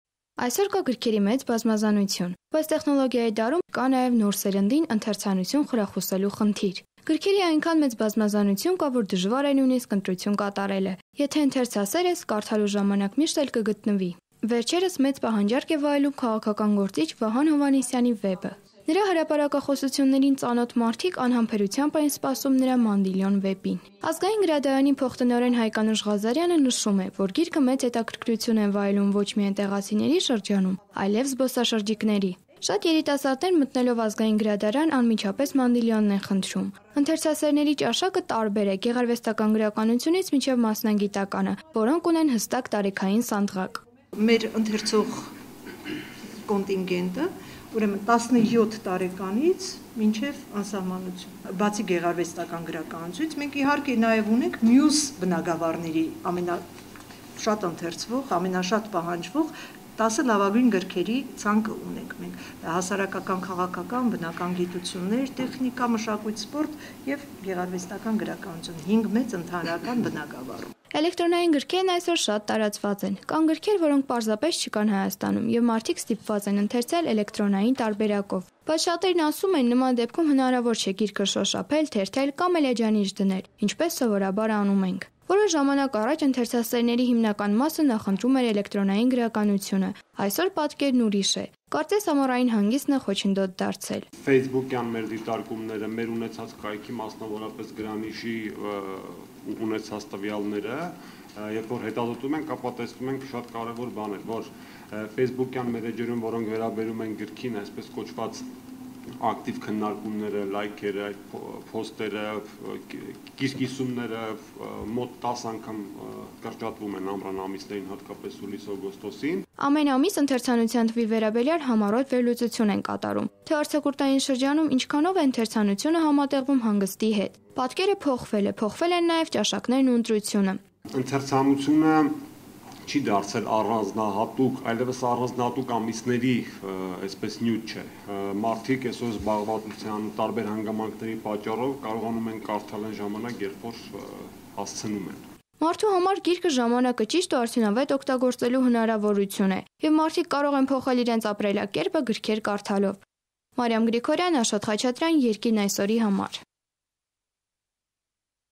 Հովհաննիսյանի «Մանդիլիոնը» մեծ հետաքրքրություն է վայելում
Այսօր կա գրքերի մեծ բազմազանություն, բայց տեխնոլոգիայի դարում այդ բազմազանության մեջ կա նաեւ նոր սերնդին ընթերցանություն խրախուսելու խնդիր: